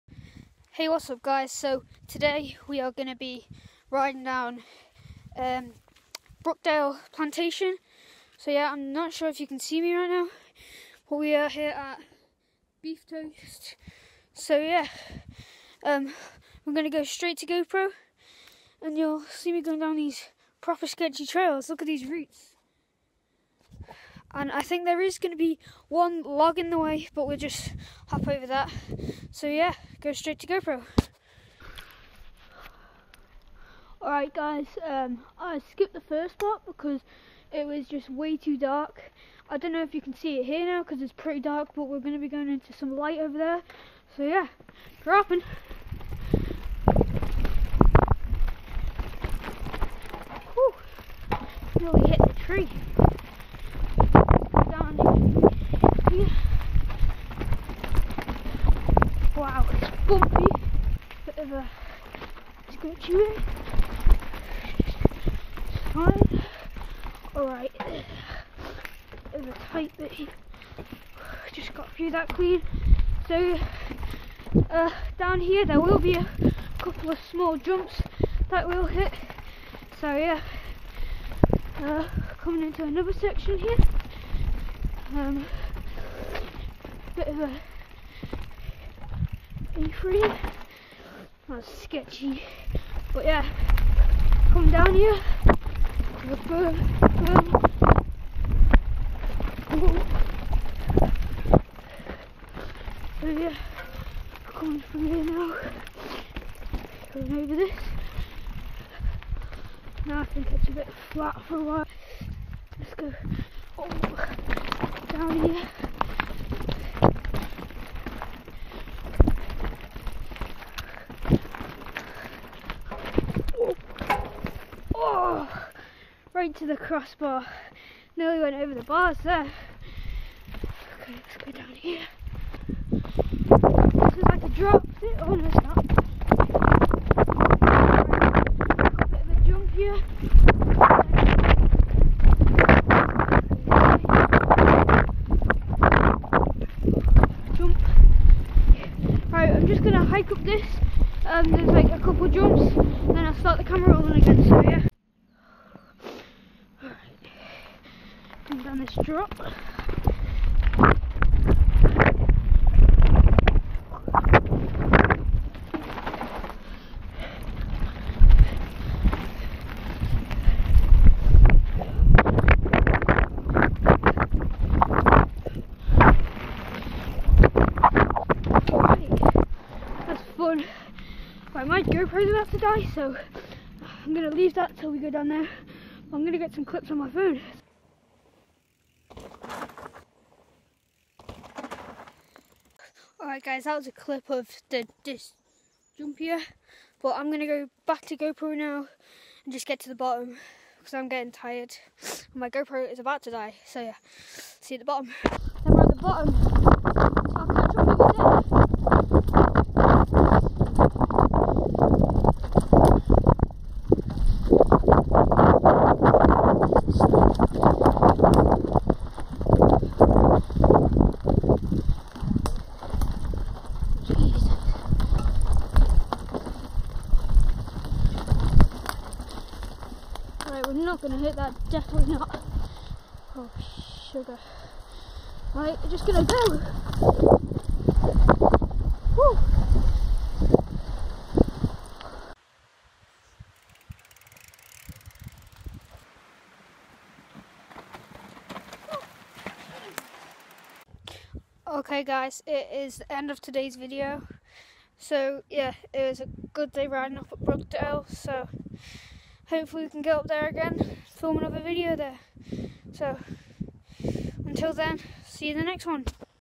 brookdale plantation isle of man!
hey guys I am gonna be going up to my local very soon to build my own trails so there will be a whole series on that but I will also be doing other stuff so the trail build doesn't get boring but remember as they say NO BUILD NO RIDE!